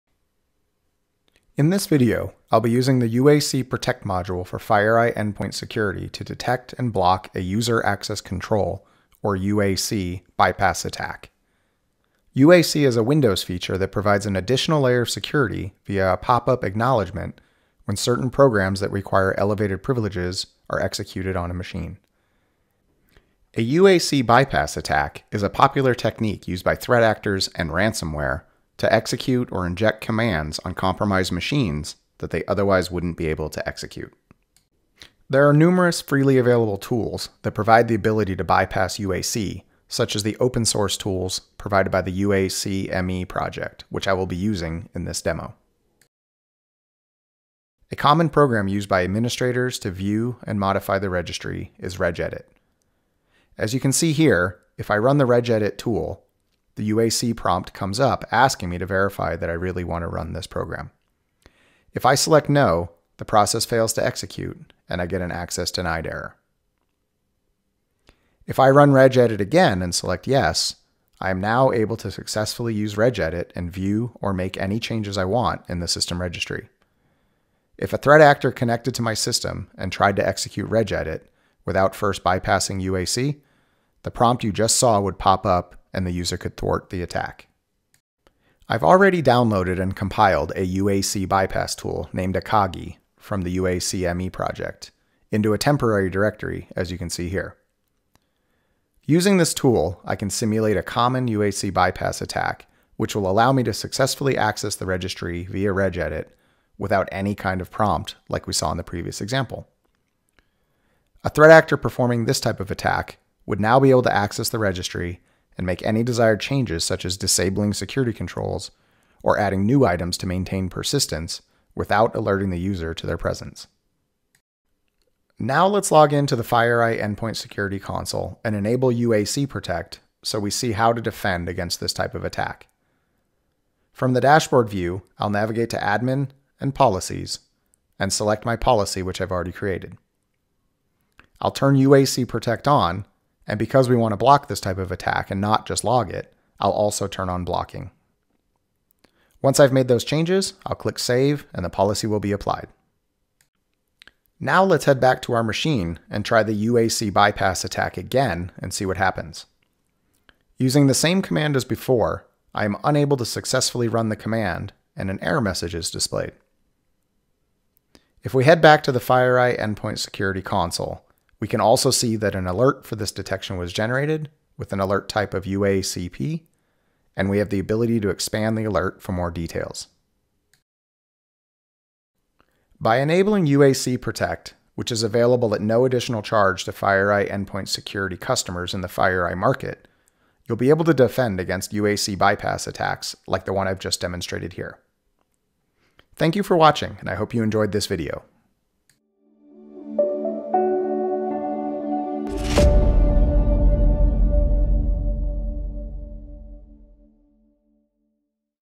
Protecting against UAC Bypass Attacks with the FireEye Endpoint Security UAC Protect Module
See how the UAC Protect module for FireEye Endpoint Security can be used to detect and block a User Account Control, or UAC, bypass attack. UAC is a Windows feature that provides an additional layer of security via a pop-up acknowledgement when certain programs that require elevated privileges are executed on a machine. A UAC bypass attack is a popular technique used by threat actors and ransomware to execute or inject commands on compromised machines that they otherwise wouldn’t be able to execute.